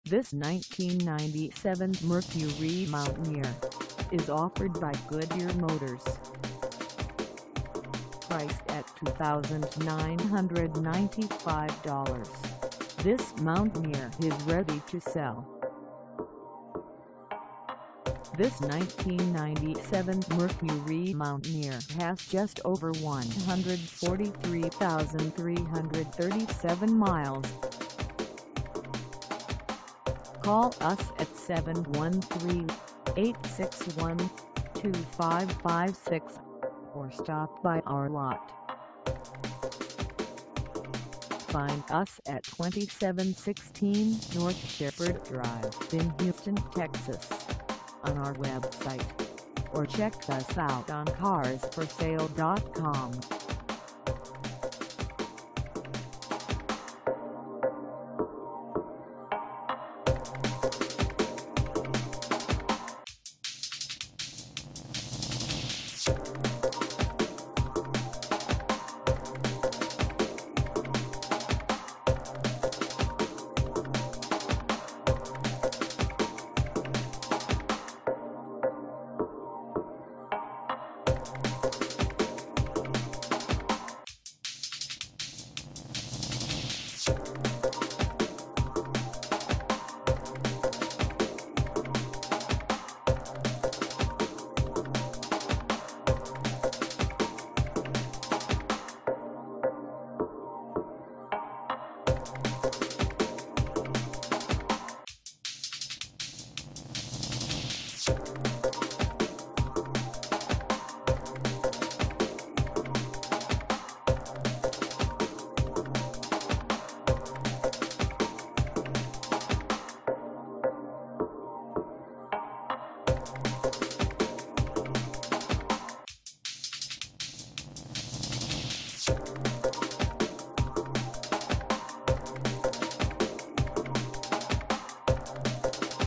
1997 Mercury Mountaineer - for sale in HOUSTON, TX 77008
Good Year Motors
2716 N SHEPHERD DR

Come test drive this 1997 Mercury Mountaineer  for sale in HOUSTON, TX.

Don't let this 1997 Mercury Mountaineer get away! It comes with a 5.00 liter 8 CYL. engine. This one's available at the low price of $2,995. With anti-lock brakes, you can guarantee stable braking on all road surfaces. This sleek vehicle comes with beige